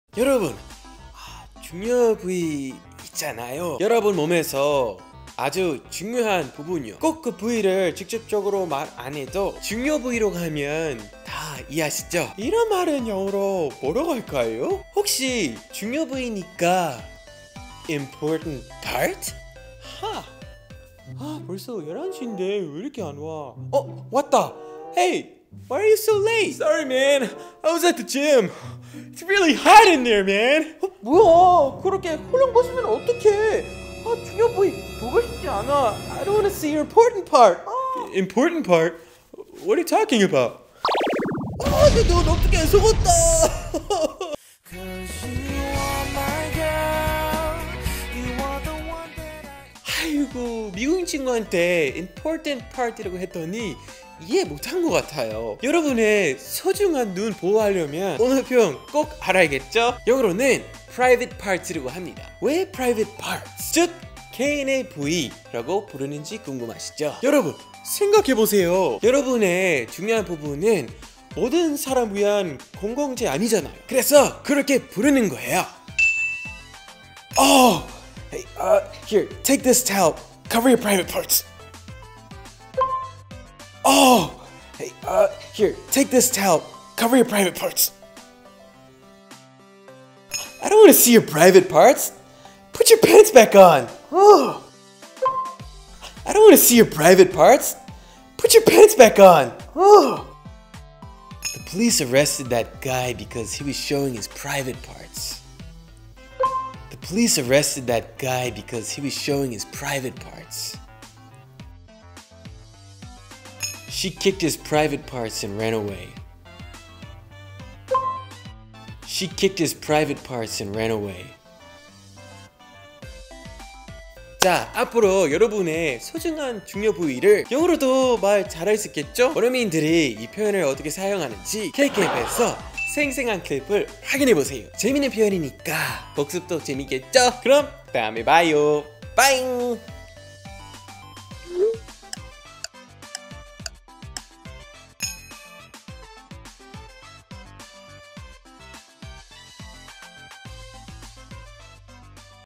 ‘중요부위’는 영어로 Important part?
중요한 부위를 영어로 뭐라고 할까요?
여러분의 소중한 눈을 위해 정확한 표현을 배워봅시다!

Free Music For Your Youtube Video